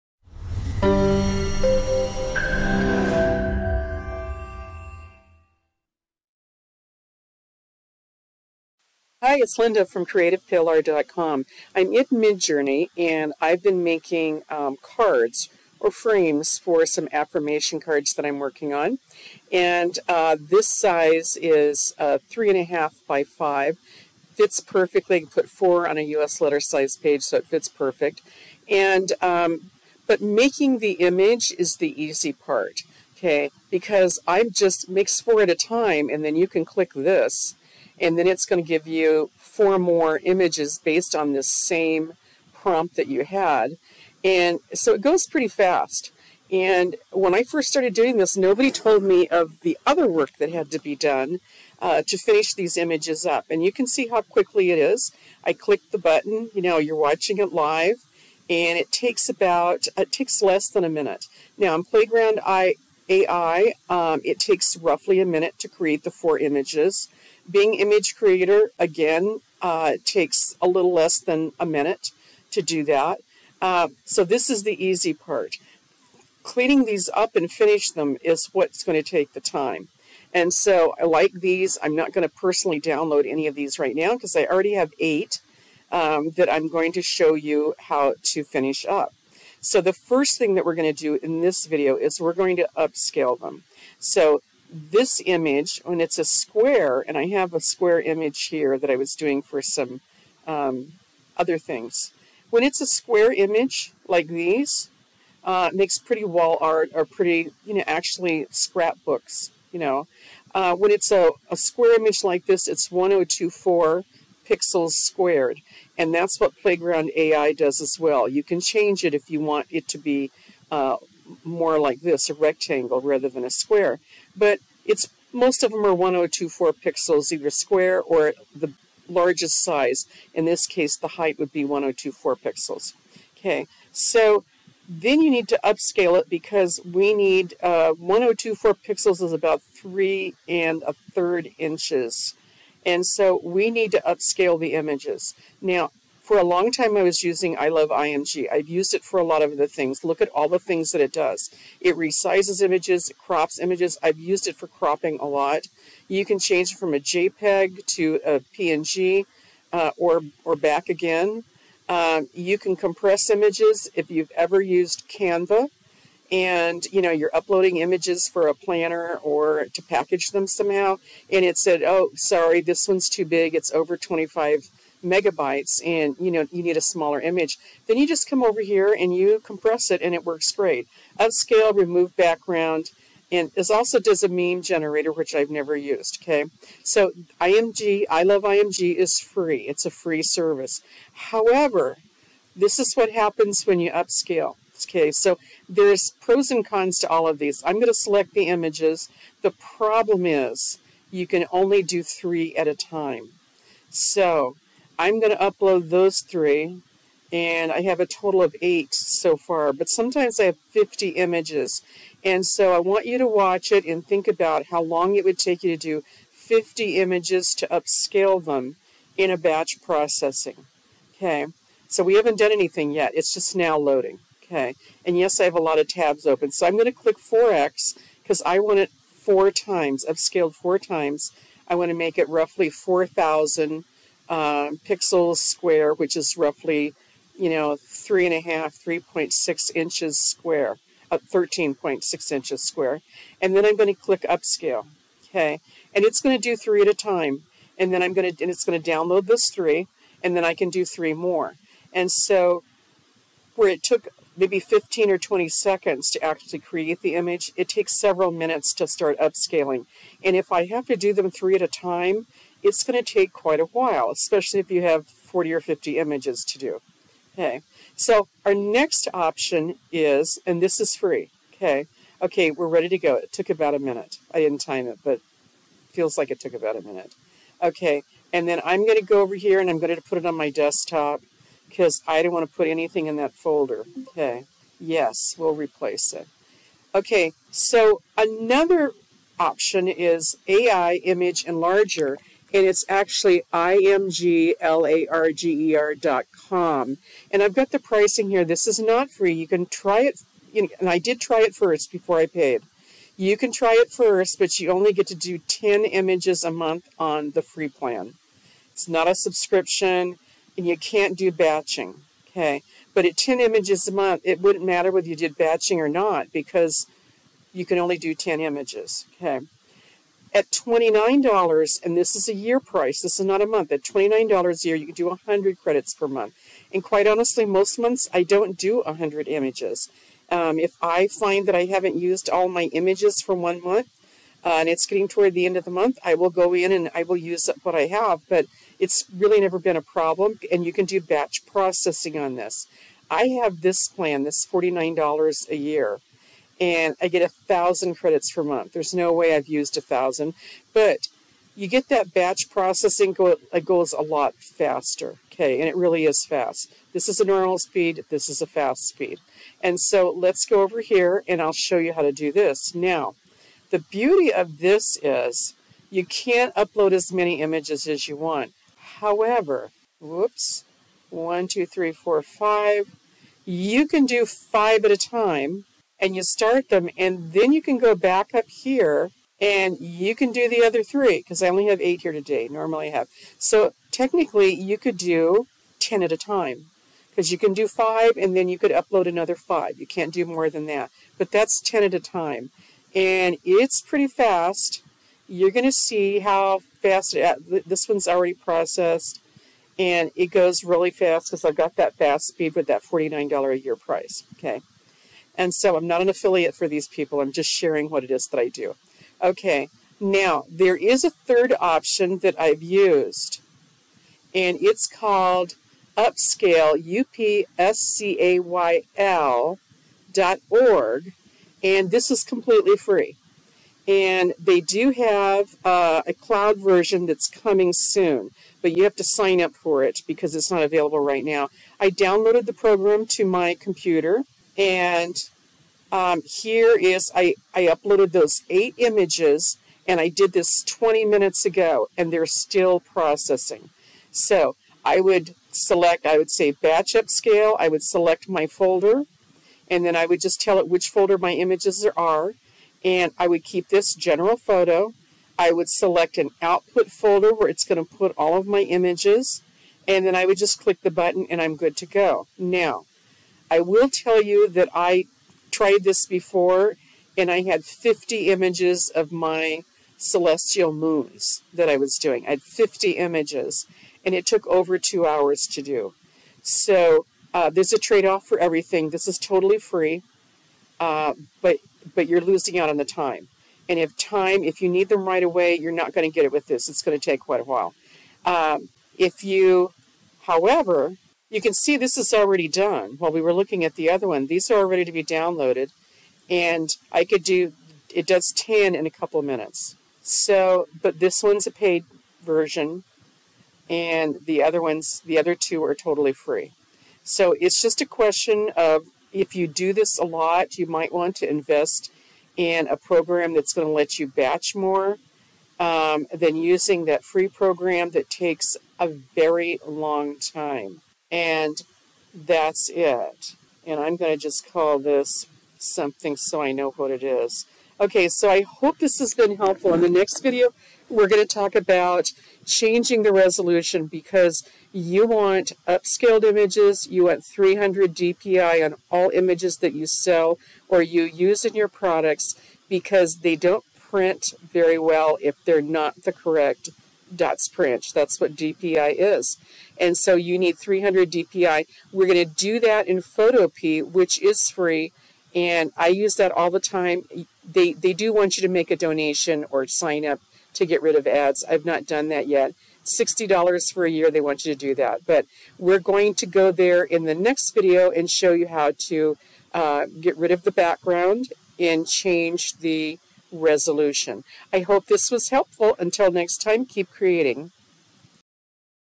Upscaling AI Images
If you are obsessed with creating AI images like me, you will need to make sure you upscale your images. Now many AI generators allow you to upscale an image 2x or 4x but to upscale one image at a time can be time consuming. You will want to batch the images to get them upscaled faster. Here I compare a few tools that will do the job.